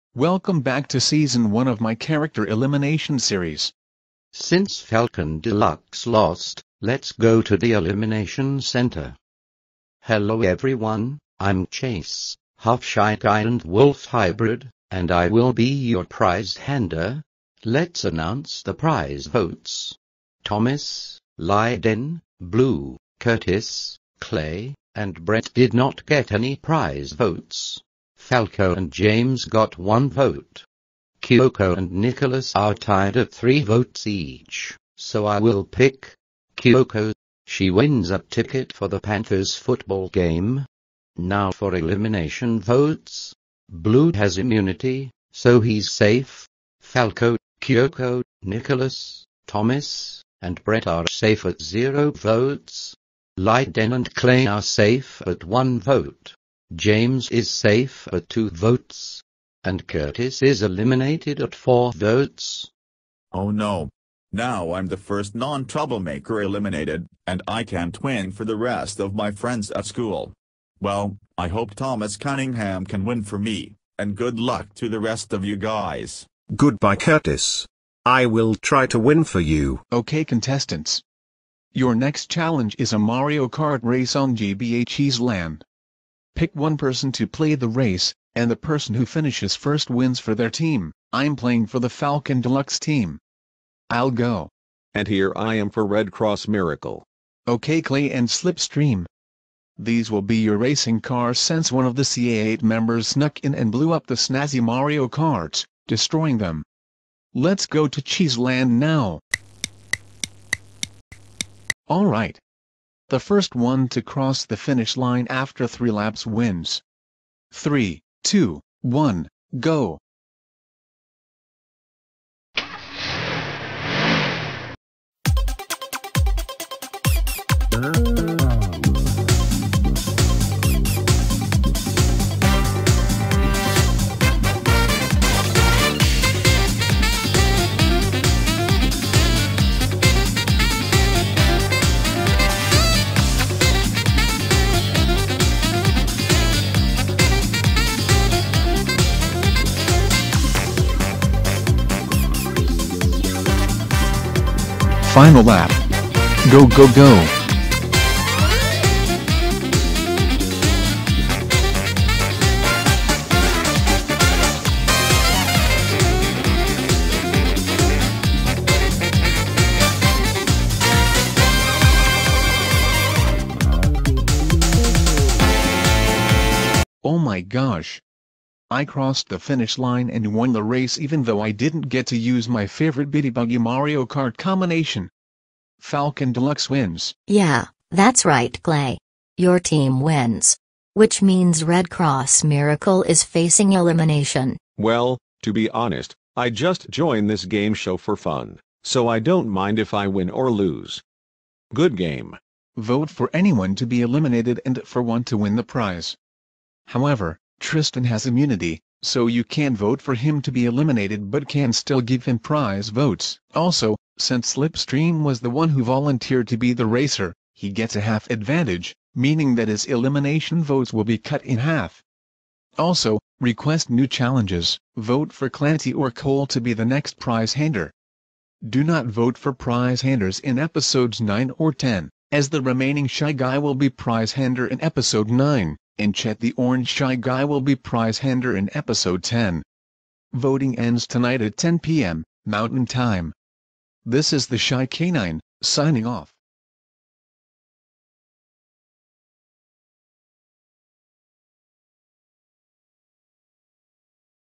Character Elimination Episode 8: Cheddar Speeds (VOTING CLOSED)
Two contestants have been selected to compete in a racing duel for a chance to win for their team.